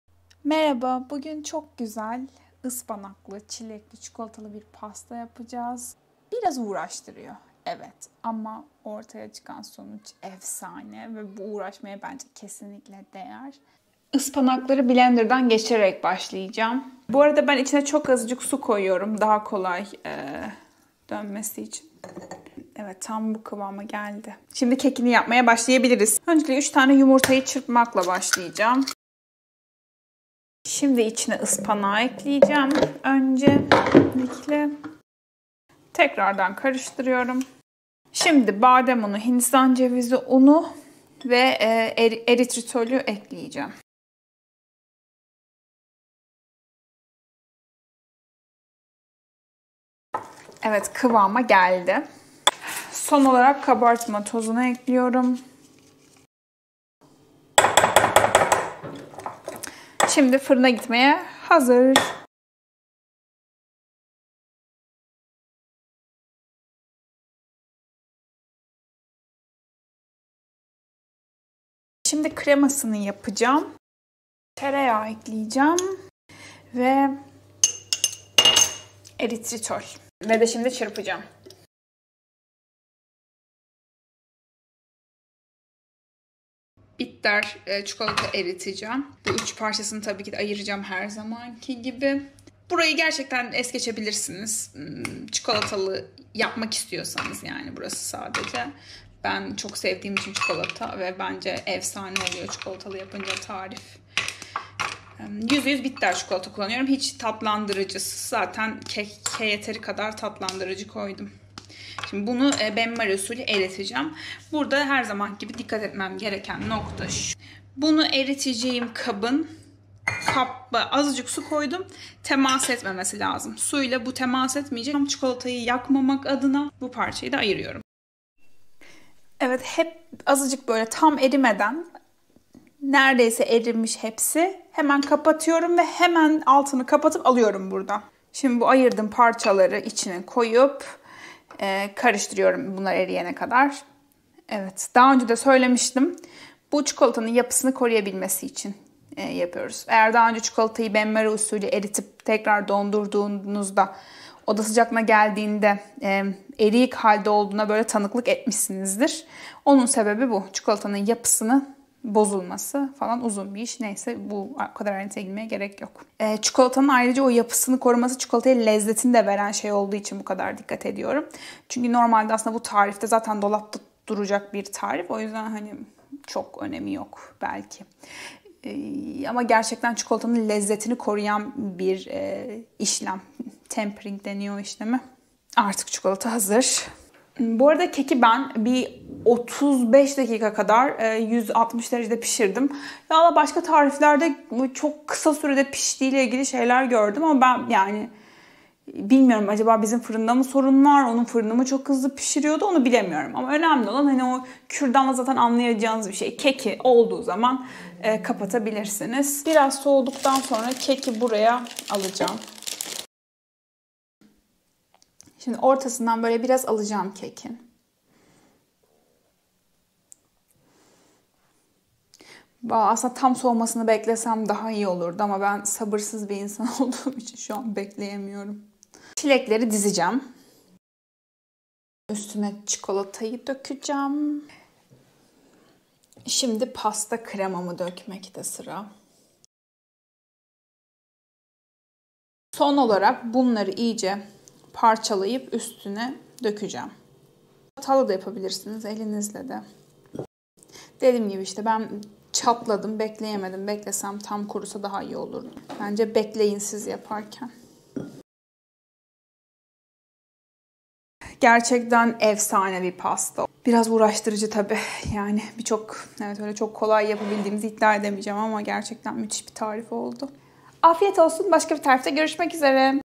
Ketojenik Ispanaklı Çikolatalı Çilekli Pasta | Ketojenik Tarifler
Malzemeler
Keki için
200 gr Ispanak
3 yumurta
110 gr / 1 + 1/2 su bardağı badem unu
35 gr  / 1/3 su bardağı Hindistan cevizi unu
60 gr / 5 yemek kaşığı Eritritol
Kabartma tozu

Pastanın içi için
15-18 Çilek
80 gr şekersiz bitter çikolata

Kreması için
200 ml krema
50 gr / 3.5 yemek kaşığı tereyağı
60 gr / 5 yemek kaşığı eritritol

100 gramındaki makrolar
Yağ 18.45
Net karb 4.24
Protein 5.56
Kalori 211.82